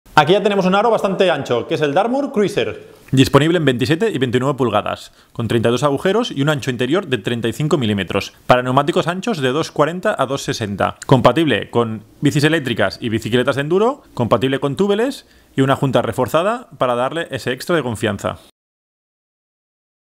Llanta Dartmoor Cruiser | Ancho interior 35mm | 27.5" y 29"
Llanta ancha de 32 agujeros para neumáticos de 2.4 a 2.8 de ancho.

Su ancho interior es de 35mm y está disponible en 27.5" y 29"

Para bicicletas eléctricas y bicicletas de Enduro MTB.